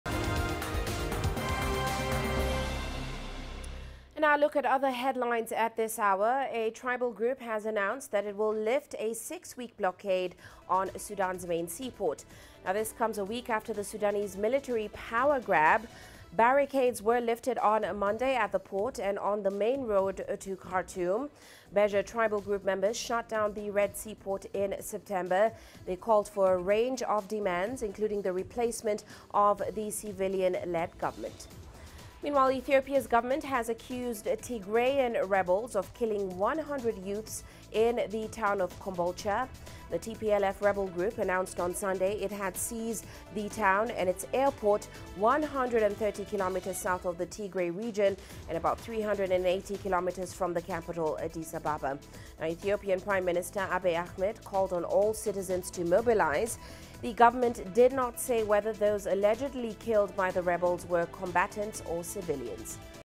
Ethiopian govt accuses Tigray rebels of killing 100 youth
The Ethiopian government has accused Tigray rebels of killing 100 youth even as a tribal group in Sudan lifts blockade on the main port.